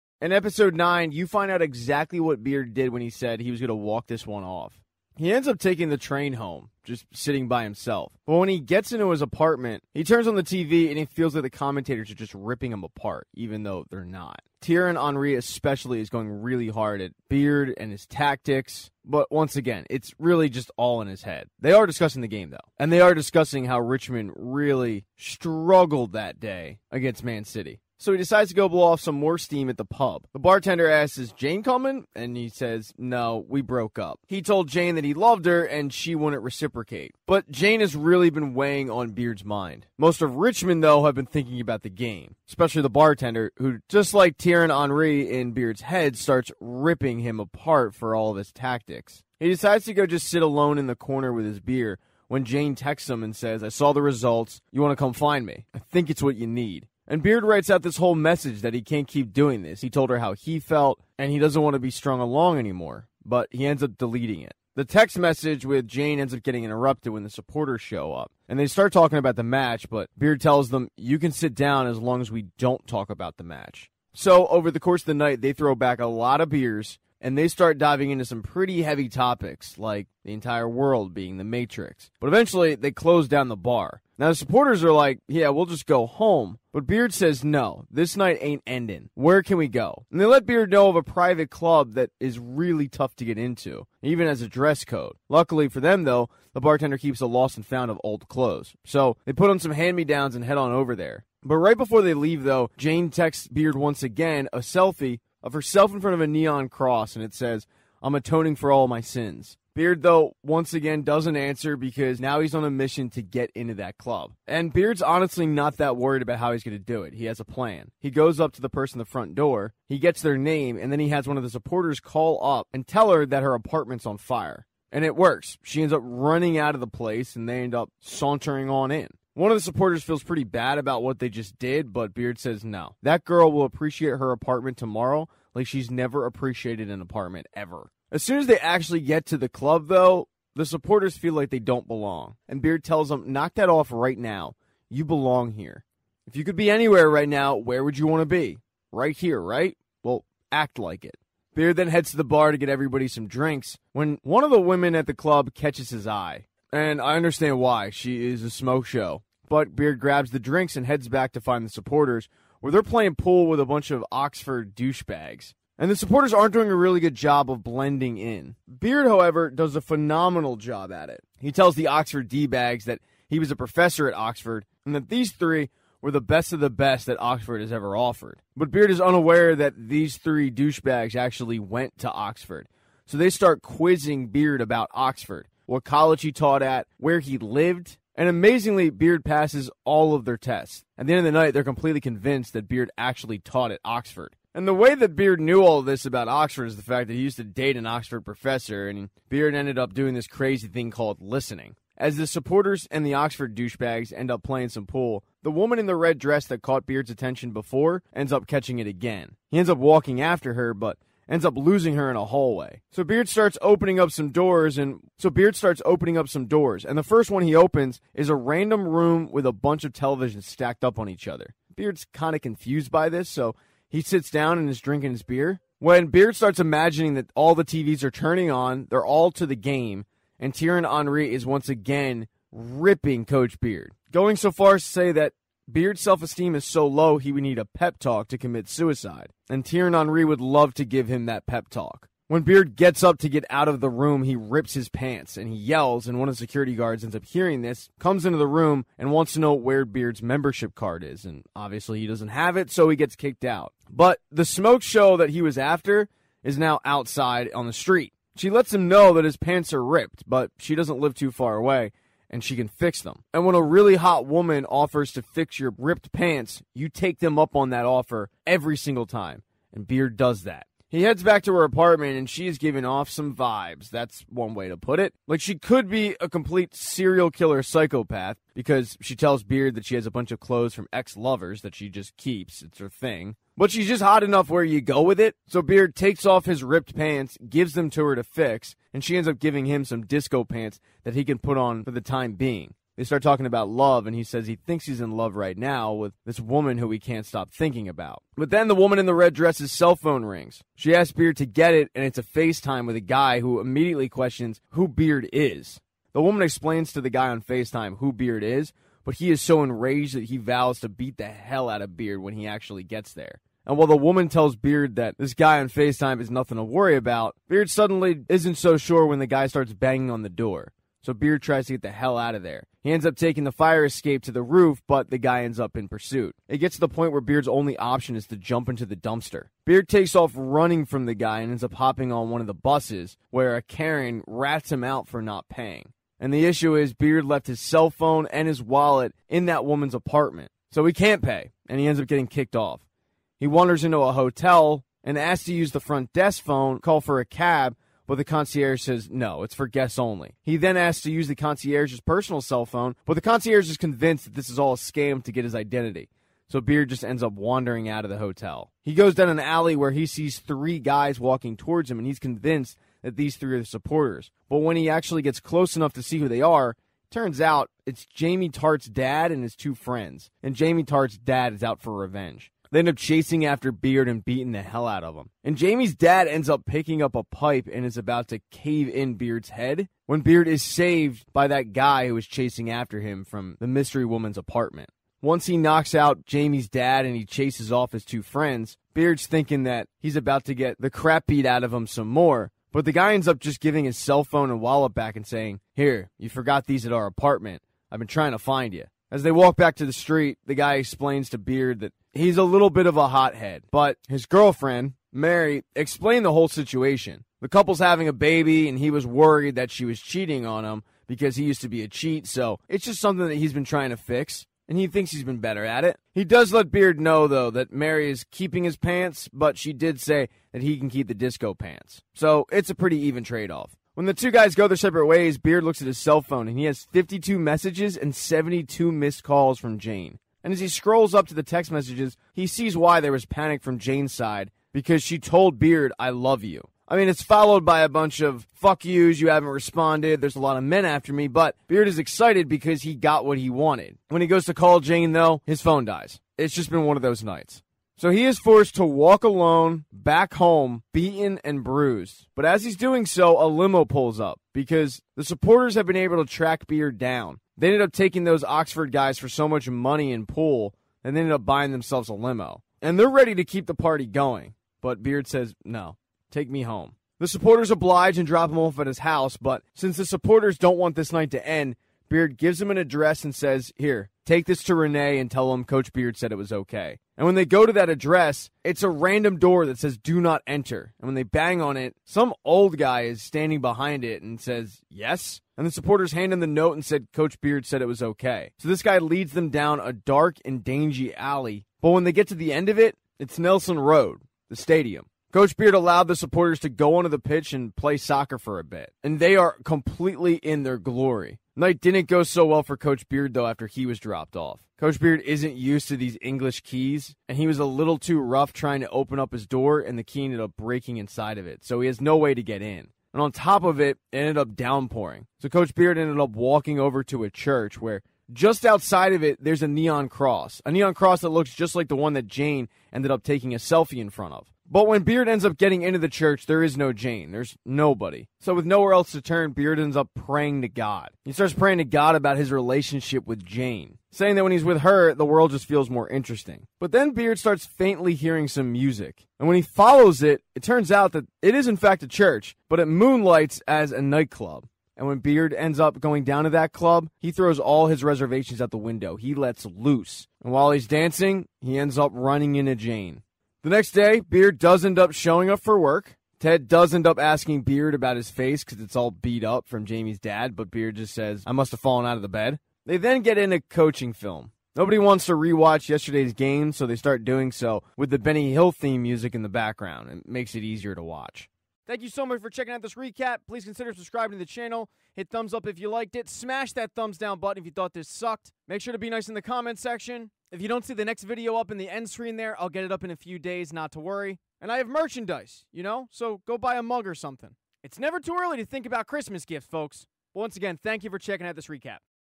Ted Lasso Season 2 Episode 9 Beard After Hours Recap
A recap of the Episode 9 of Season 2 of Ted Lasso! Beard After Hours

And check out my podcast Scene Invaders wherever you listen to podcasts